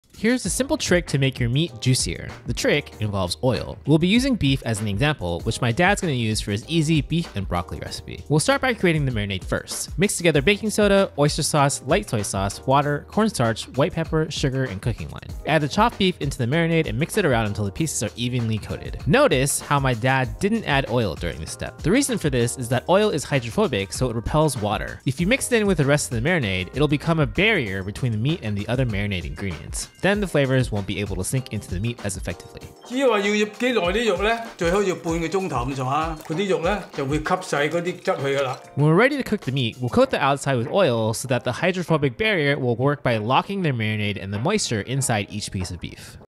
🙅🏻‍♂️ One ingredient NOT to add to marinade
Here’s a simple trick to make your meat juicier — with one key ingredient. But, make sure you’re using it correctly to make the most of your meat and marinade!

How do you like to marinate meat?